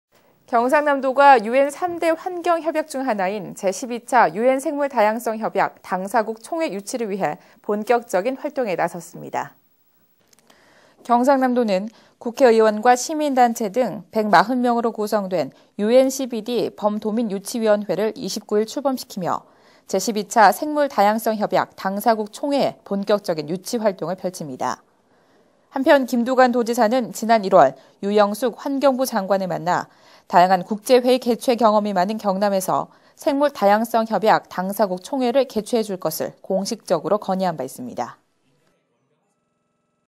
유엔생물다양성협약 당사국총회 유치 본격화
경상남도는 국회의원과 시민단체 등 140명으로 구성된
UNCBD 범도민 유치위원회를 29일 출범시키며
제12차 생물다양성협약 당사국총회의
본격적인 유치활동을 펼칩니다.

한편, 김두관 도지사는
지난 1월, 유영숙 환경부장관을 만나
다양한 국제회의 개최 경험이 많은 경남에서
생물다양성협약 당사국 총회를 개최해 줄 것을
공식적으로 건의한 바 있습니다.